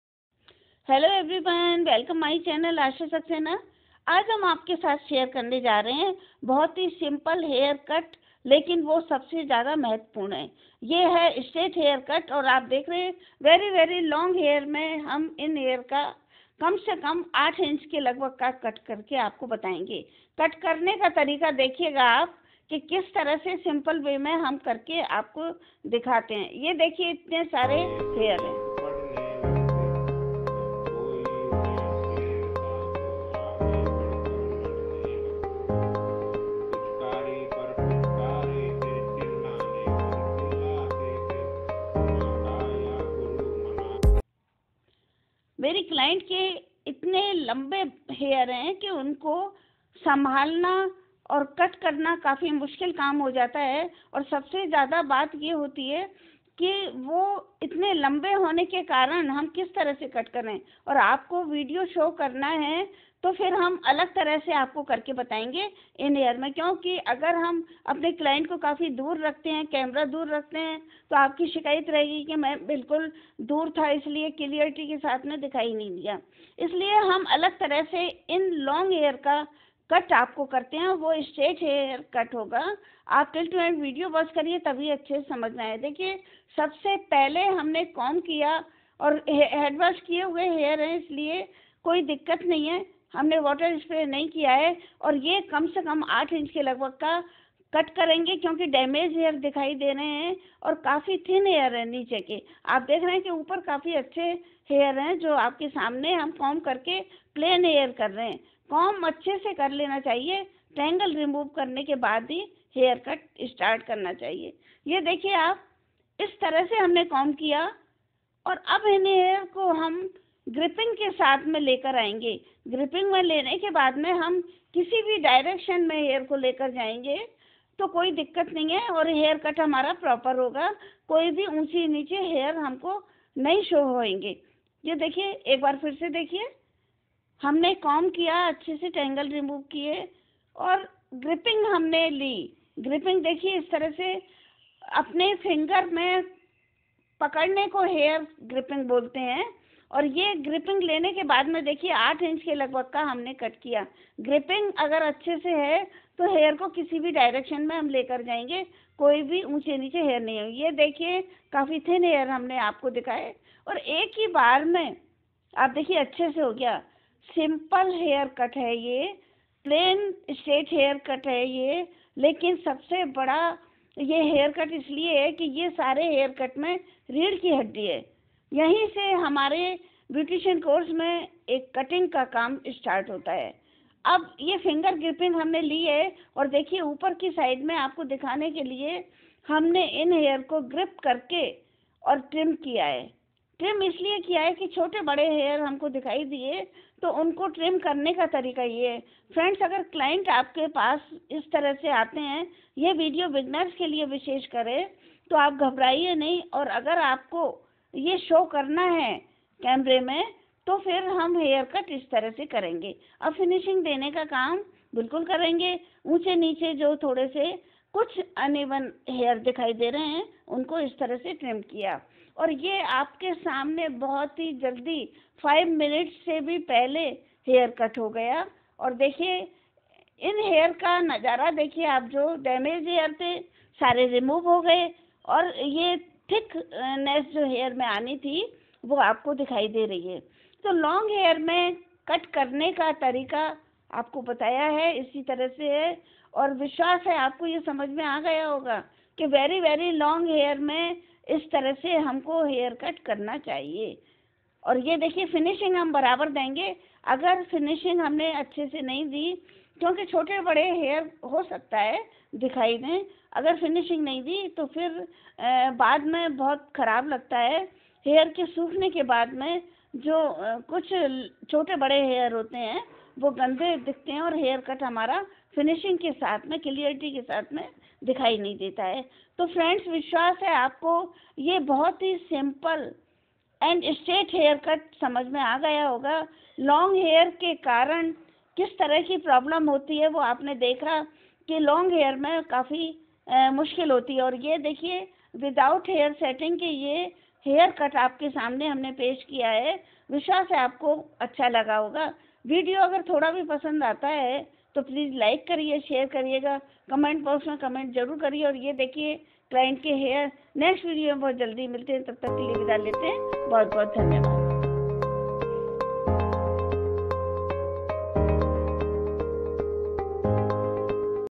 STRAIGHT HAIRCUT IN VERY VERY LONG HAIR FOR ATTRACTIVE LOOK.TUTORIAL.
Hello Friends kese hain aap sab? Aaja hum aapke liye ek useful Video lekar aaya hain Please watch my new Straight haircut.This haircut is specially designed for Beginners.So I explained in detail step by step in easy way.So that one can learn it easily.Thanks.🙏
ASHA SAXENA BOMBAY BEAUTY PARLOUR JAIPUR RAJ.